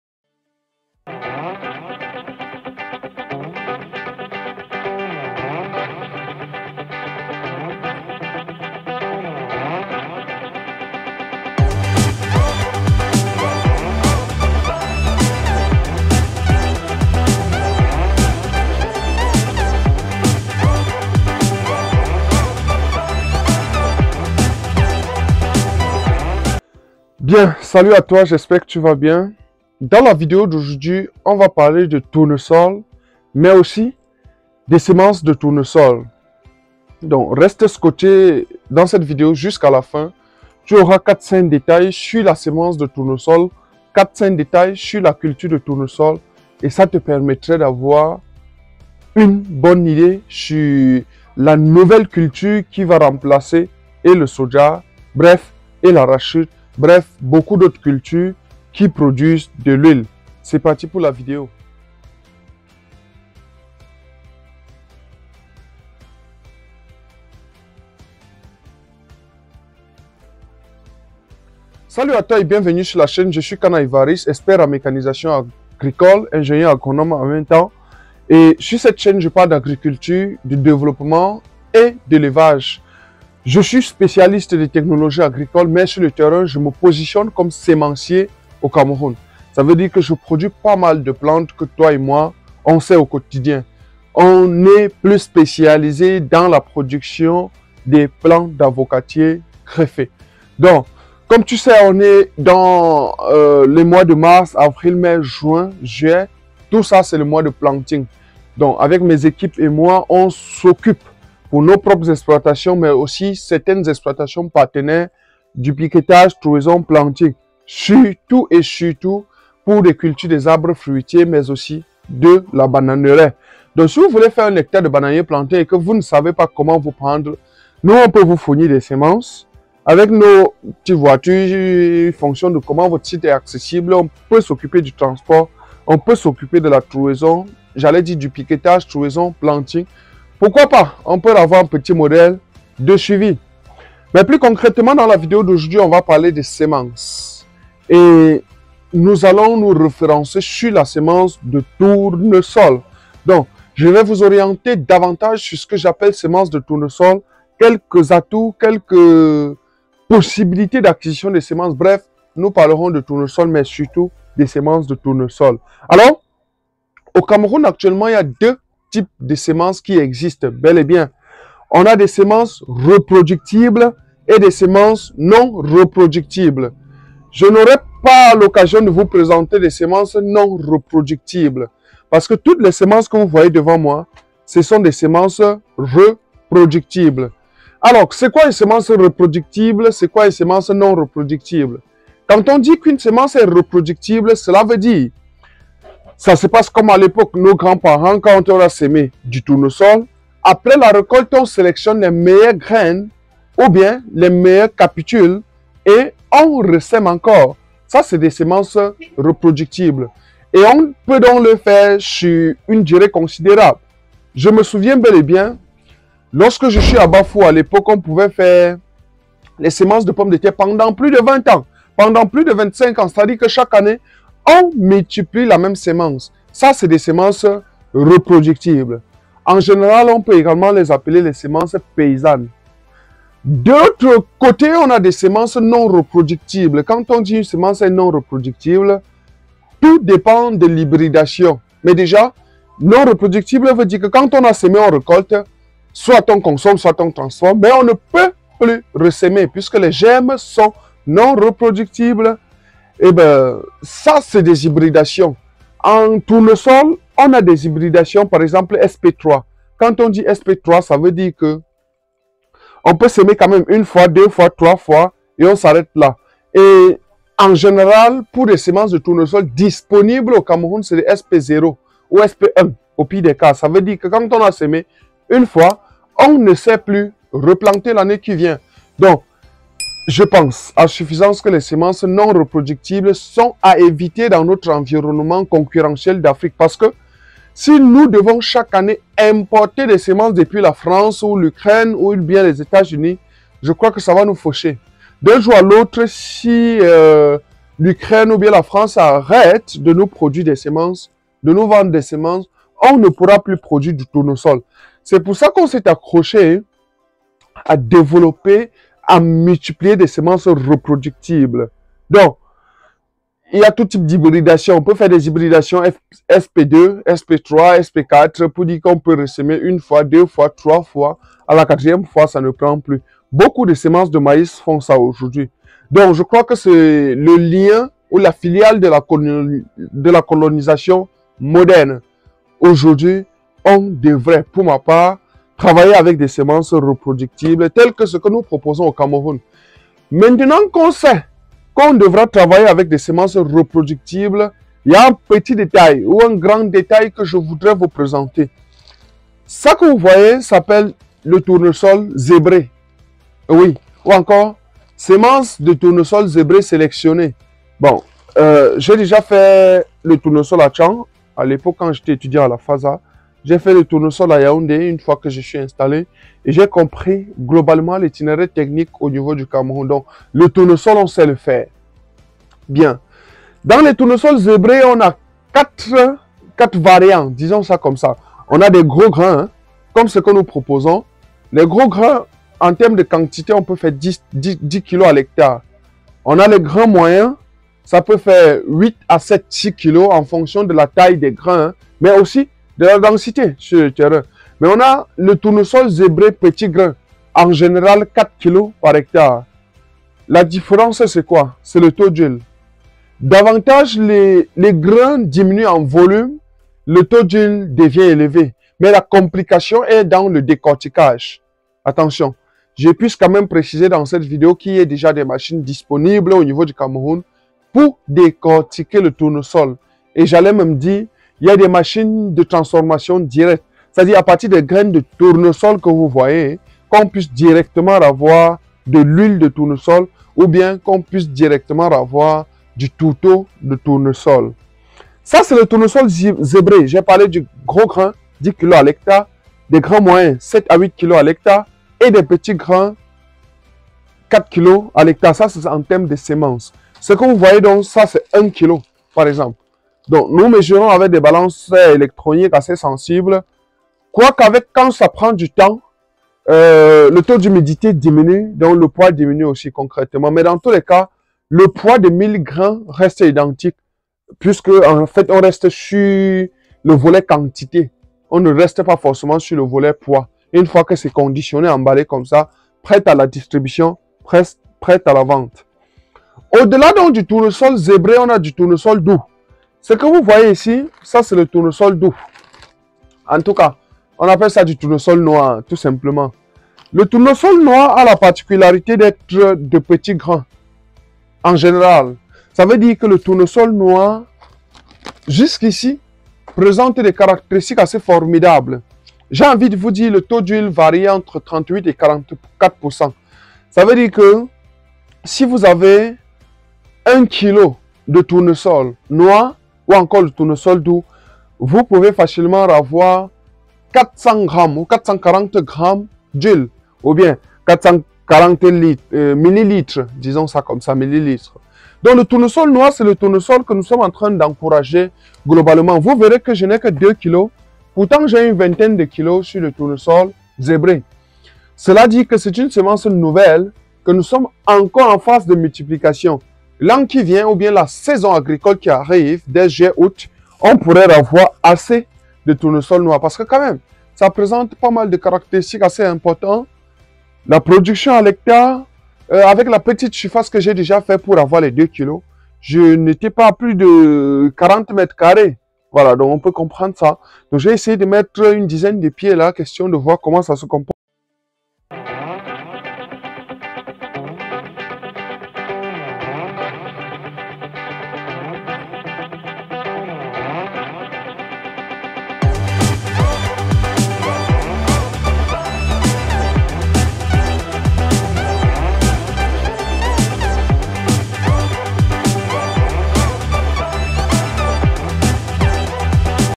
MEILLEURE SEMENCE DE TOURNESOL
le tournesol également connu sous les noms hélianthe et soleil, est une grande plante annuelle, appartenant a la famille des ASTERACEES ,dont les fleurs sont groupées en capitules de grande dimensions.
Les graines et huile de tournesol sont des excellente source de vitamine E;
les graines de tournesol sont riches en acide gras insaturés..